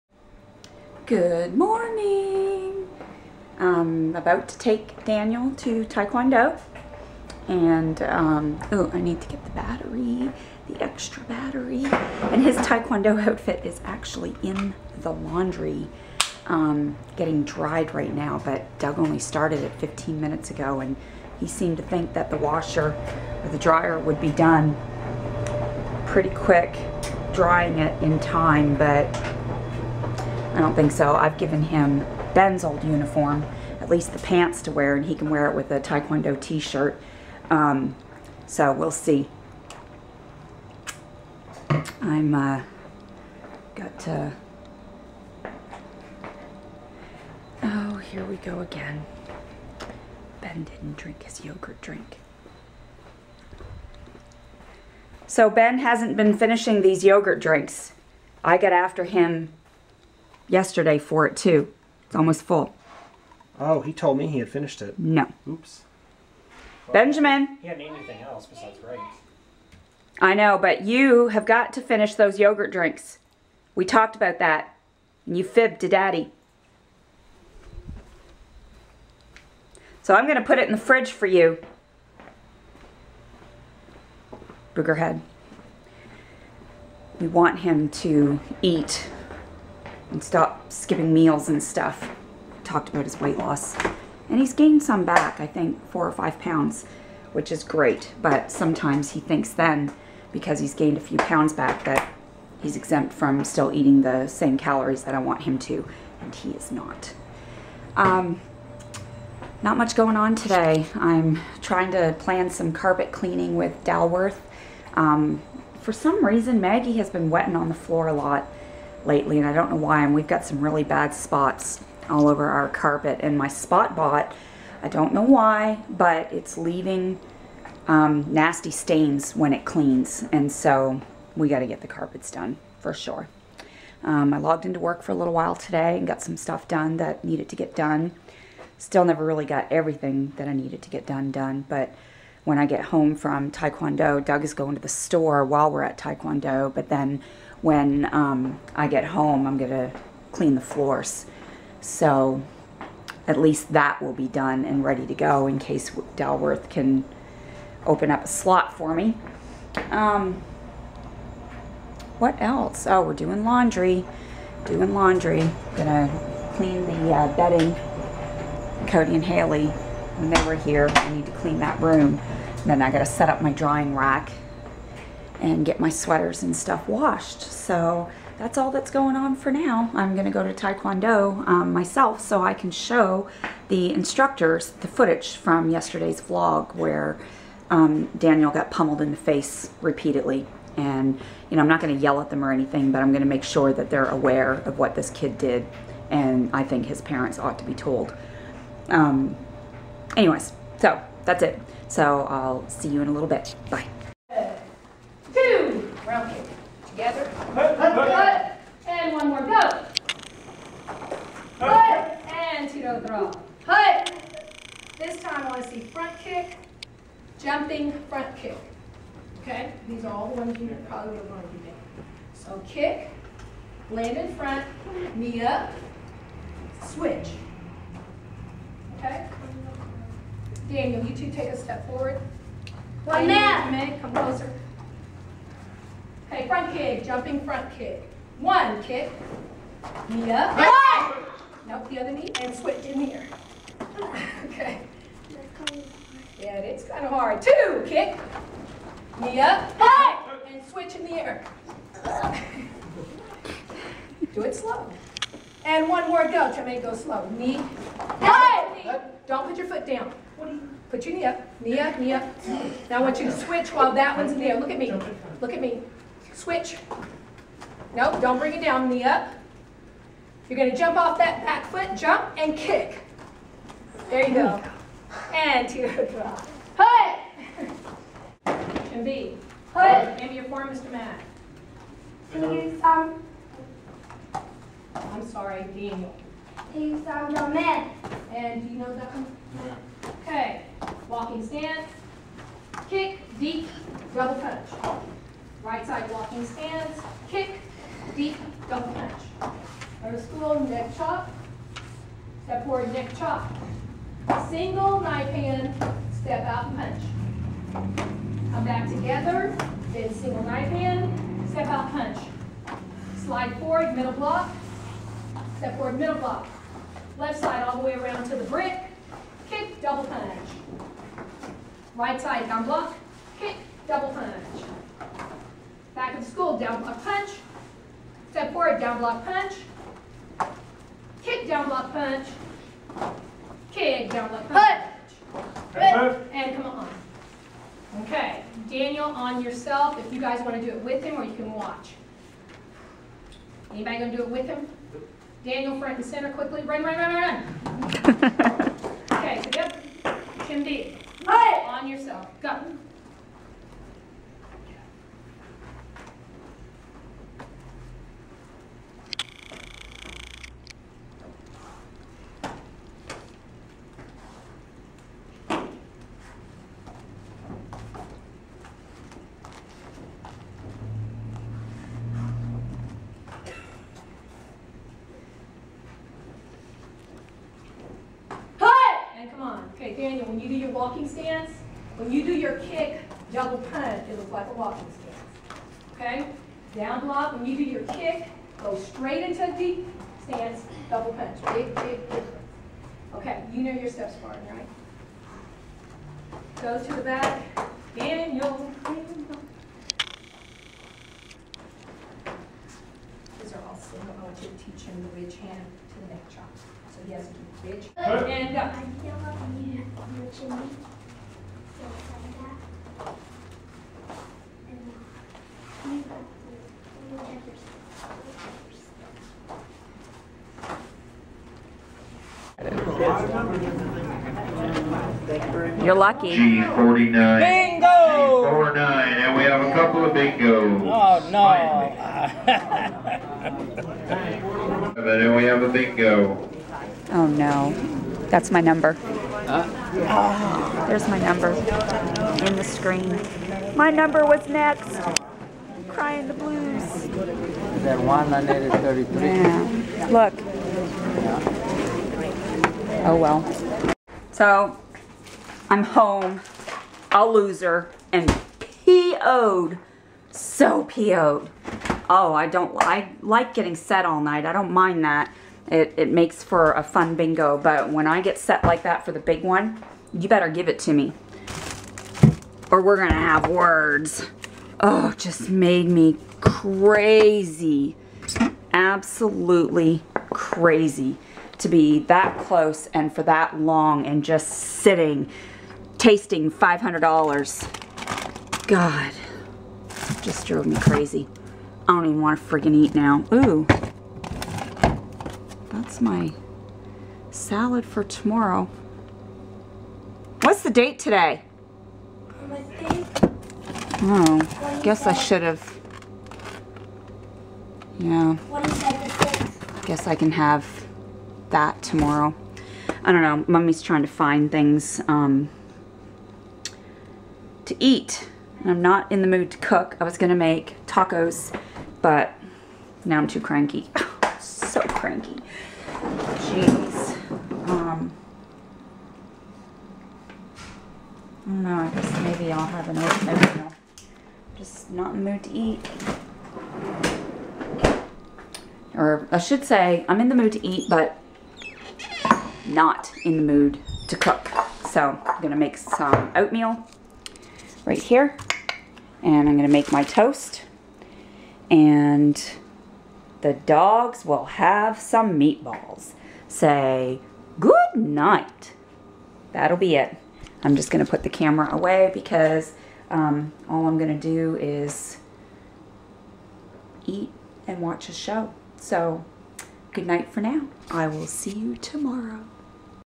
Vlog - Oct 6, 2013
Another day in the life of the Ingram household. I started vlogging after my surprise visit home to Canada to see family! Thought they'd enjoy seeing more of my life here in TX.